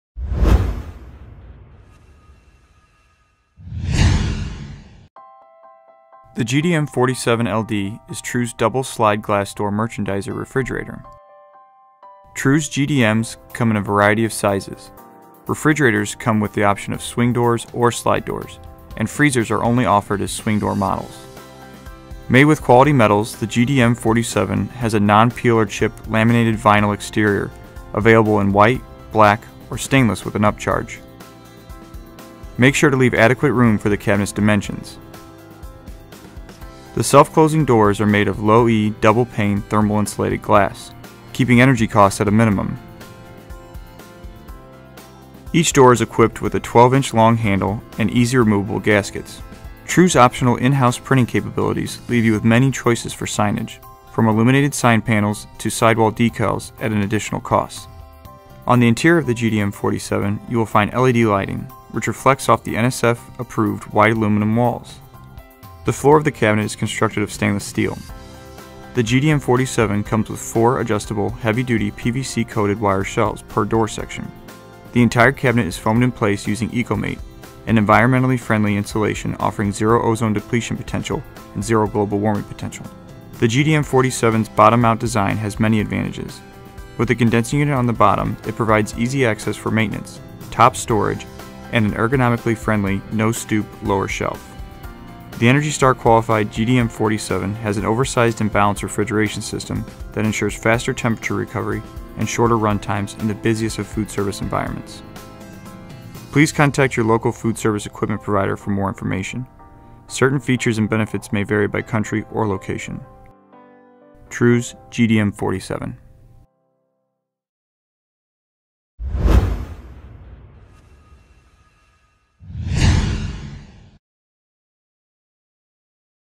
True® GDM-47-LD Merchandising Refrigerator | Glass Door, Double Slider | Convenience Stores
This video highlights the many advantages of True GDM-47-LD merchandising refrigerators. These commercial units come in a variety of sizes and are ideal for displaying cold beverages.

"True GDM-47-LD Merchandising Refrigerator | Glass Door, Double Slider | Convenience Stores" Video Transcript:

The GDM-47-LD is True's double slide glassdoor merchandiser refrigerator. True's GDMs come in a variety of sizes. Refrigerators come with the option of swing doors or slide doors. And freezers are only offered as swing door models. Made with quality metals, the GDM-47 has a non-peeler chip laminated vinyl exterior--available in white, black, or stainless with an upcharge. Make sure to leave adequate room for the cabinet's dimensions.

The self-closing doors are made of low e double pane thermal insulated glass--keeping energy costs at a minimum. Each door is equipped with a 12-inch long handle and easy removable gaskets. True's optional in-house printing capabilities leave you with many choices for signage. From illuminated sign panels to side wall decals at an additional cost. On the interior of the GDM-47, you will find LED lighting--which reflects off the NSF approved white aluminum walls. The floor of the cabinet is constructed of stainless steel. The GDM-47 comes with 4 adjustable heavy-duty PVC-coated wire shelves per door section. The entire cabinet is foamed in place using eco made--an environmentally friendly insulation offering zero ozone depletion potential and zero global warming potential.

The GDM-47's bottom mount design has many advantages. With the condensing unit on the bottom, it provides easy access for maintenance, top storage, and an ergonomically friendly no stoop lower shelf. The Energy Star-qualified GDM-47 has an oversized and balanced refrigeration system that ensures faster temperature recovery and shorter run times in the busiest of foodservice environments. Please contact your local food service equipment provider for more information. Certain features and benefits may vary by country or location. True's GDM-47.

Feel free to let us know what else you’d like to see in our videos by leaving a comment.